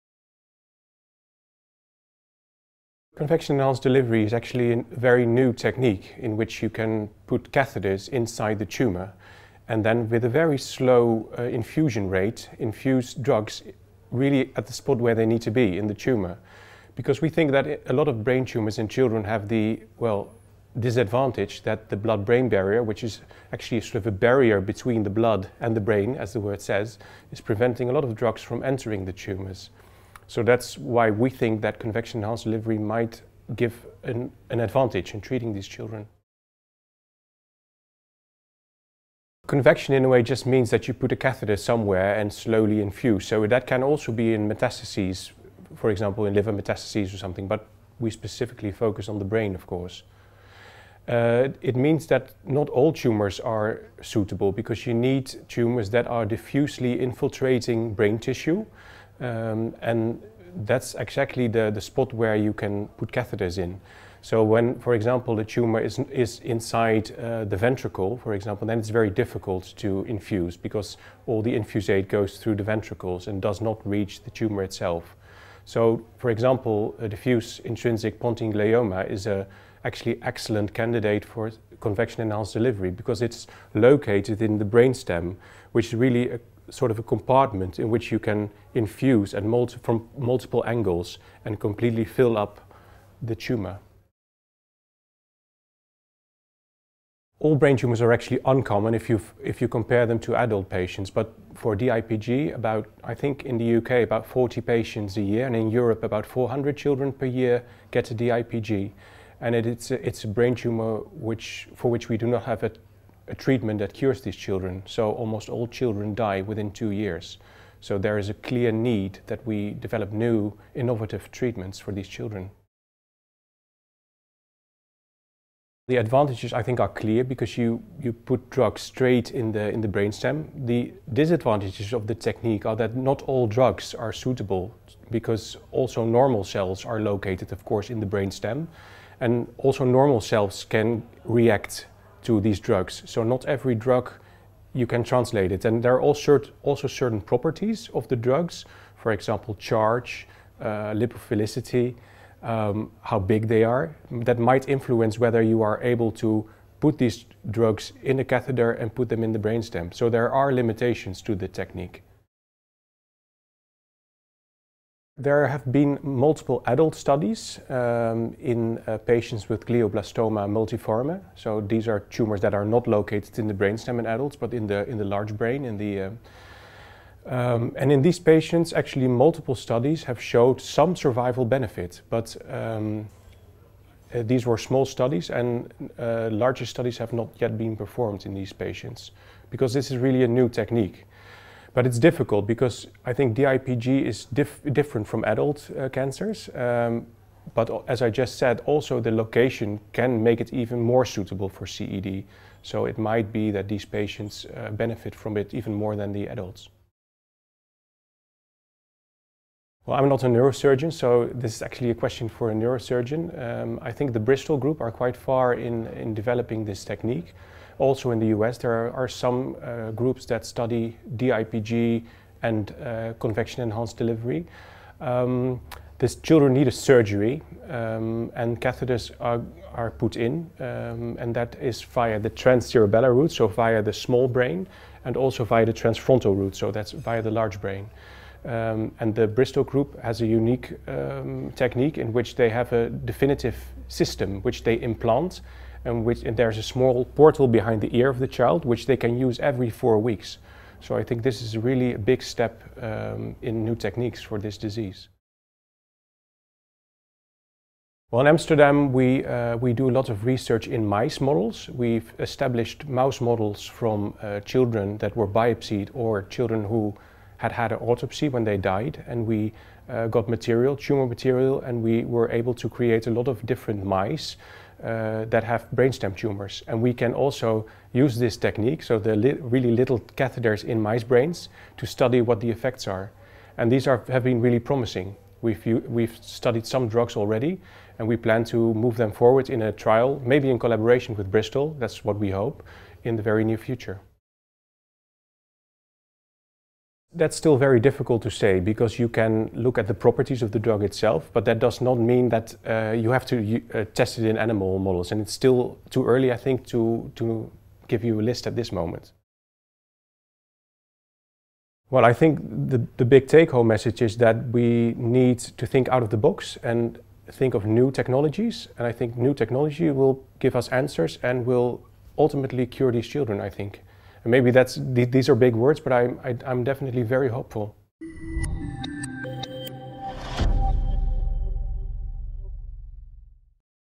Convection-enhanced delivery of drugs to brain tumours in children
Dr Van Vuurden talks to ecancertv at Children with Cancer UK’s workshop on Drug Delivery in Paediatric Brain Tumours in London, UK.

In the interview he discusses convection-enhanced delivery (CED) of drugs to brain tumours in children.

CED is a relatively new technology that requires very fine and small catheters to be implanted in the brain that can then be used to delivery chemo- or other therapeutic agents. This method of drug delivery is particularly of interest for treating children with diffuse intrinstic pointine glioma (DIPG) who currently have few treatment options and could potentially benefit from local chemotherapy in the brainstem.